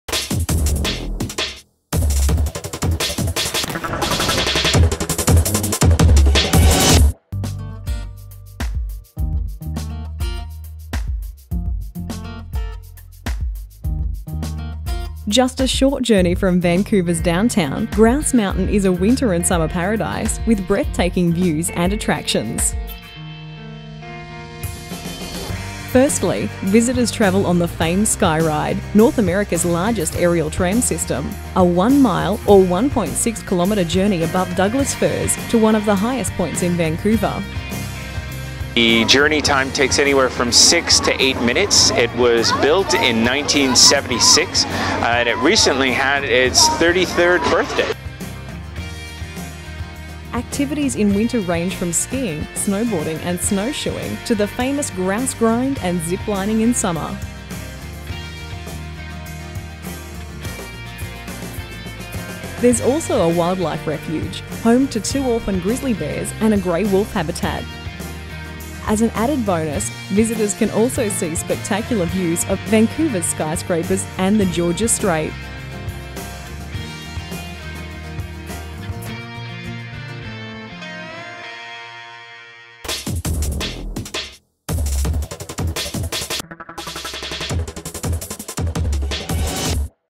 Hostelling International: Grouse Mountain
An insiders guide to Vancouver's Grouse Mountain